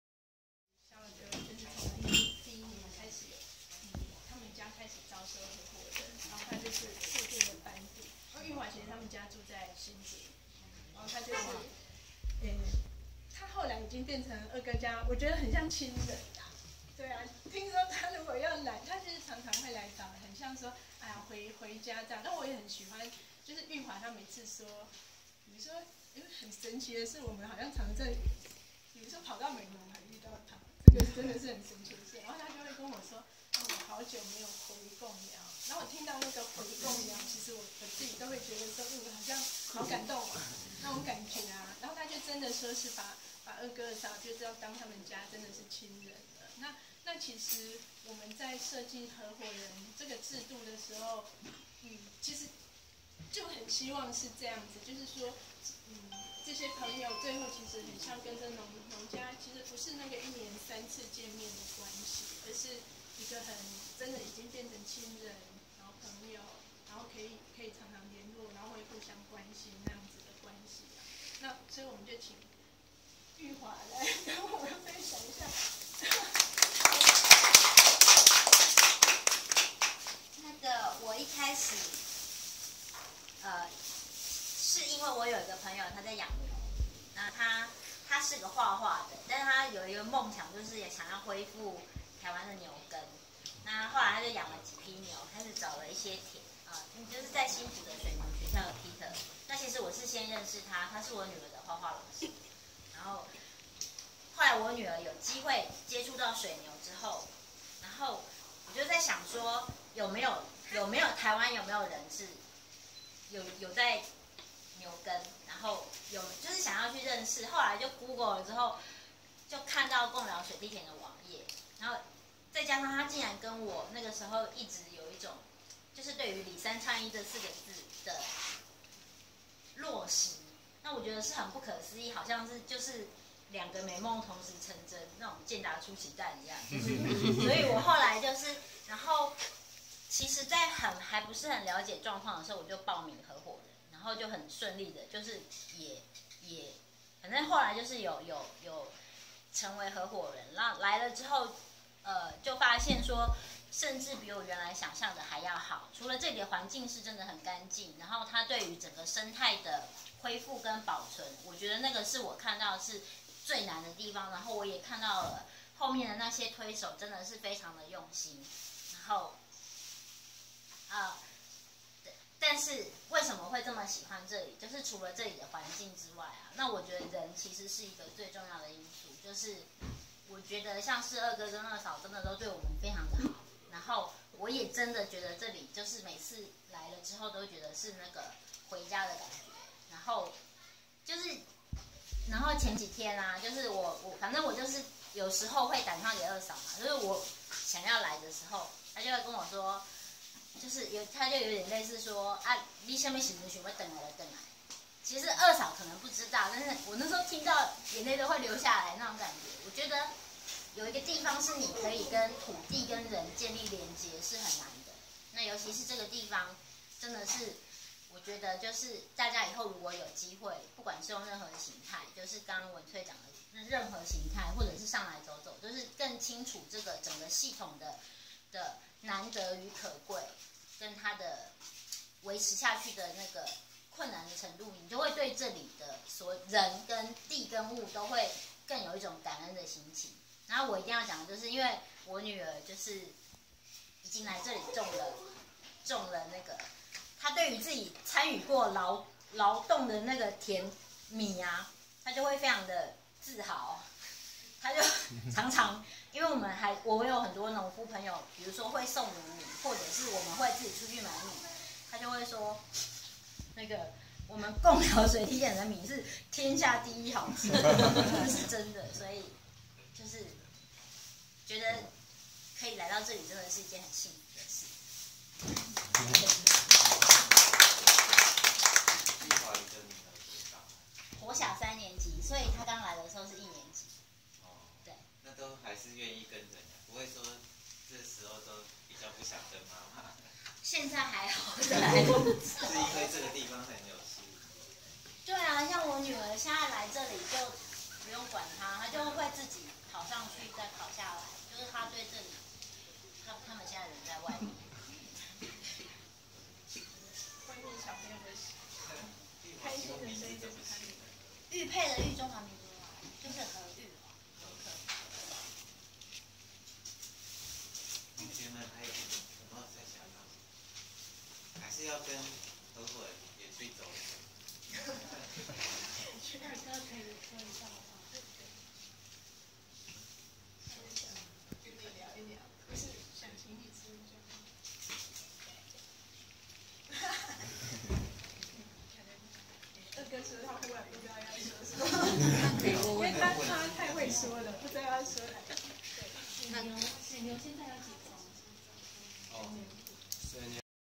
和夥人玉華說貢寮有回家的感覺，有土地和人可以連結是很難得的，貢寮的米是天下第一好吃MOV 0080
田邊聊寮聊『和禾生產班』貢寮山區現場實作（二）